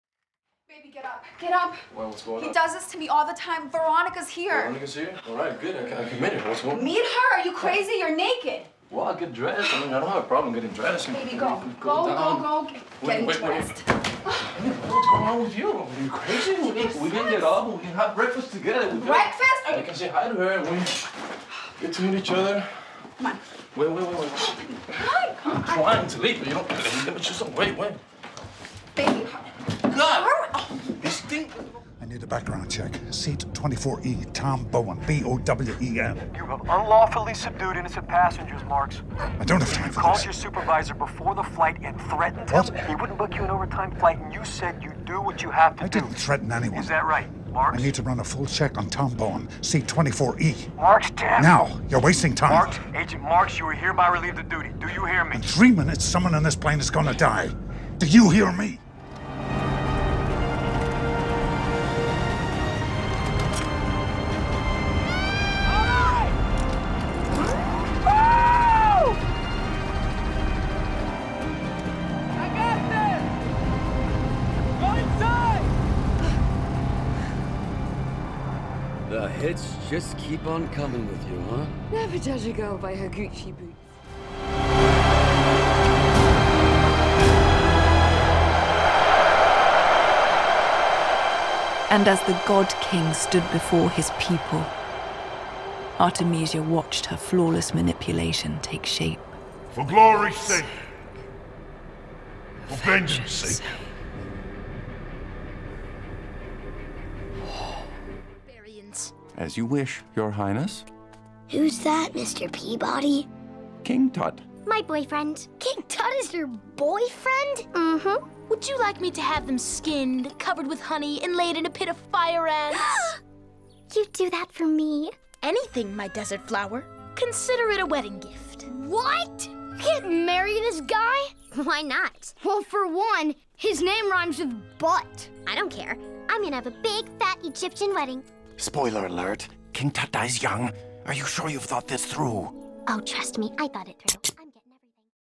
Weekend Box Office - March 14 - March 16, 2014 - Studio Earnings Report HD
Weekend Gross

1. Mr. Peabody & Sherman - $21.2 million
2. 300: Rise Of An Empire - $19.1 million
3.
5. The Single Moms Club - $8.3 million